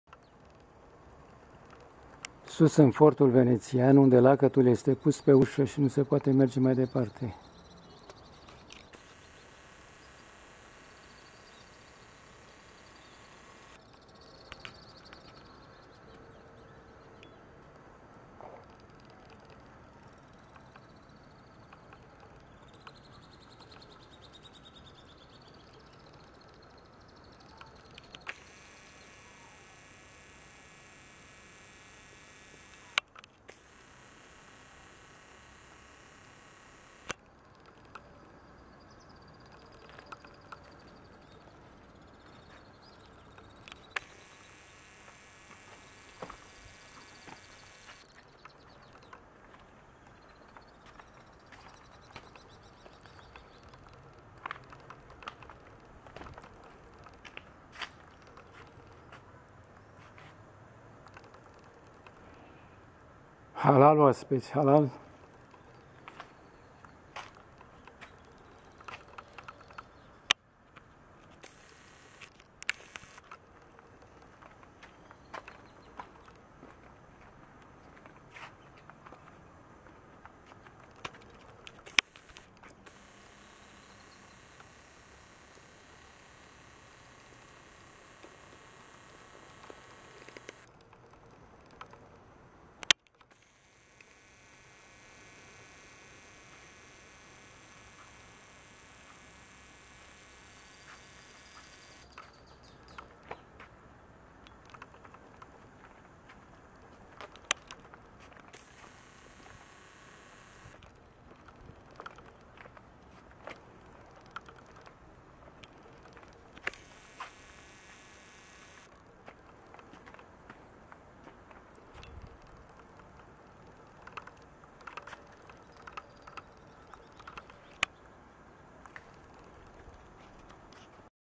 Corfu Town: In Old Fortress (4)
As it is surrounded by the sea, the fortress was in fact used both to defend Corfu town, and to check the near coast. Inside, one can see the two fine towers built by the Venetians, the Castel del Terre, 64 meters in height, and the Castel del Mare, 51 meters high, together with the ruins of the gun batteries, underground tunnels, cisterns, barracks, powder magazines and other buildings dating from Venetians and British times.Inside the fortress there are different buildings such as a prison built in 1786 by Venetian and made it bigger in a second moment by English. Three vaulted corridors lead to the other areas. Two elaborated stone stairways lead to the tallest level of the Fortress where there is polygonal water well decorated with embossed representations, coats of arms and a sign of the 18th century
Not far from the prison, can also visit the military church called "Madonna of Carmini" and an ancient military hospital converted today into a music school.